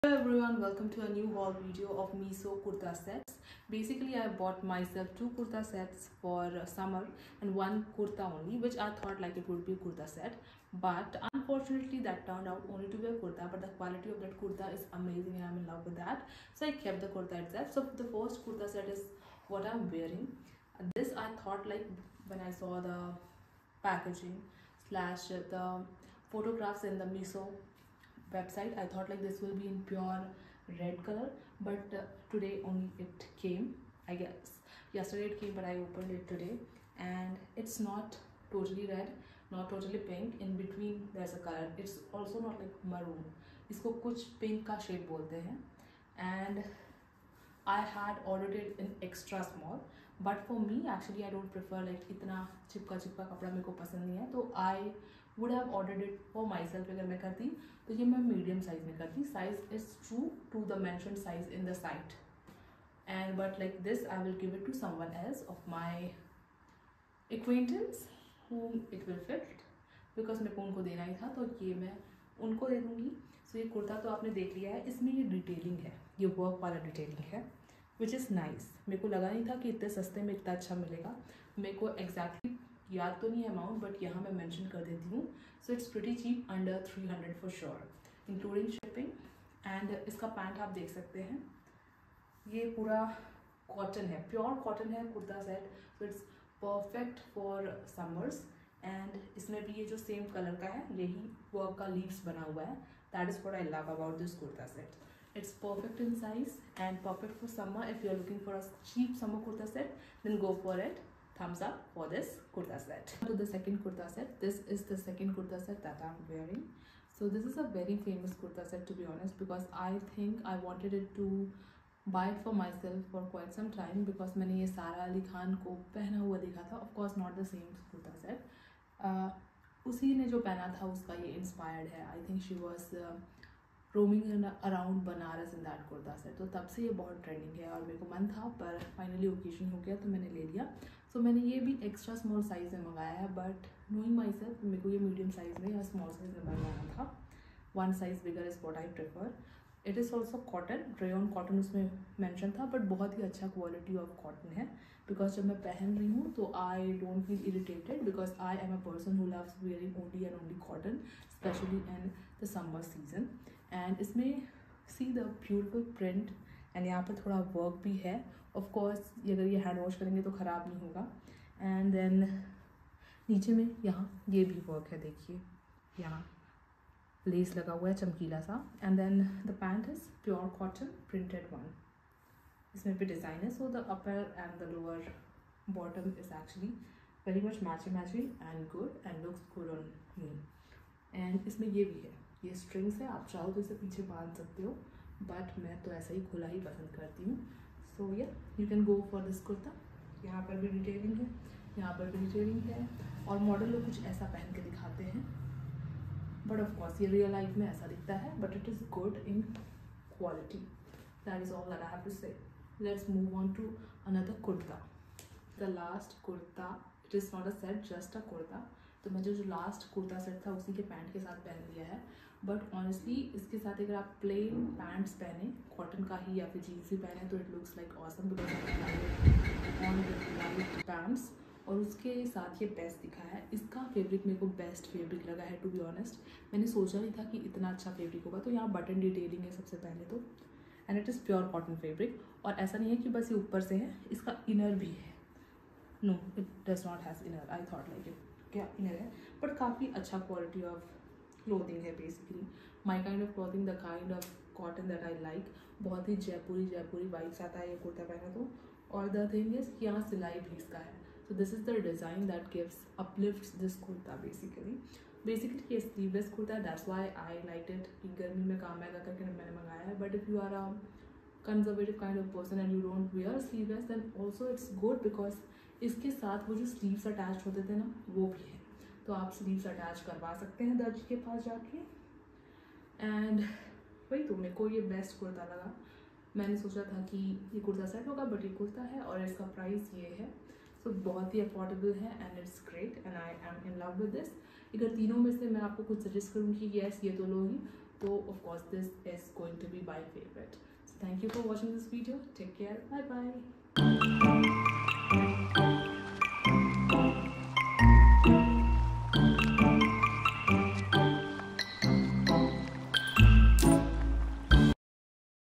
Affordable Cotton Kurta Sets from Meesho for Summer | Under Rs. 500 Daily Wear Lookbook 🌺
Looking to update your summer wardrobe on a budget? Check out this try-on haul of affordable cotton kurta sets from Meesho!

In this video, I'll be showing you how I styled these Meesho kurta sets for a relaxed, casual look that's ideal for the summer season. I'll also share my thoughts on the quality, fit and value of these budget-friendly Indian ethnic wear finds.

Whether you're looking to refresh your work wardrobe or just want some easy, breezy outfit options, these Meesho kurta sets have got you covered! Don't forget to let me know your favorite looks.

Magenta Pink Kurta set : Out of stock